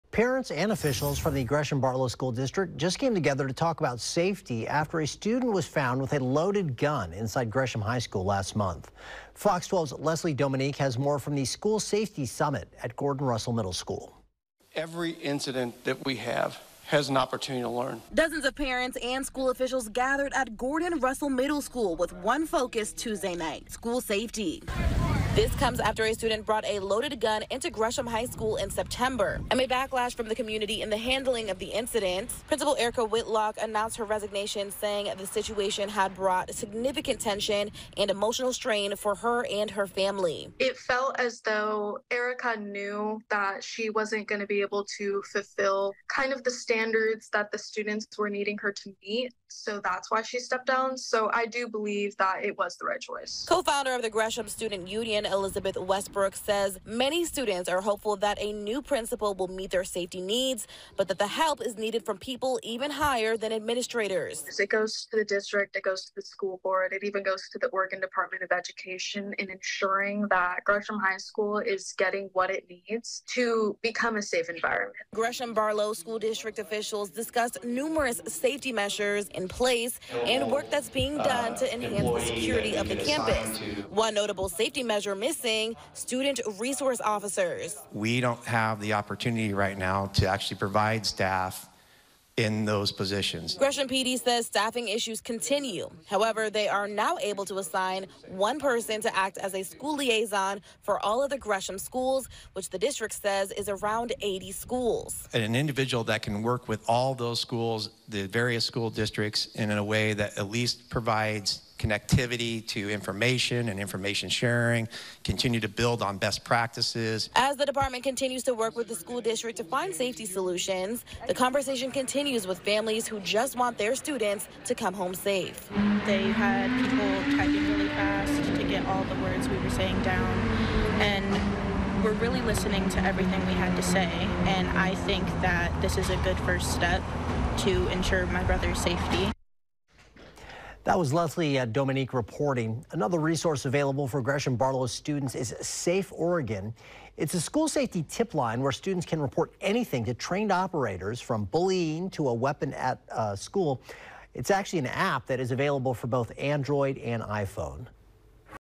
Gresham-Barlow School District hosts 'School Safety Summit' following safety concerns
The Gresham-Barlow School District hosted a “School Safety Summit” Tuesday night following threats to Gresham High School.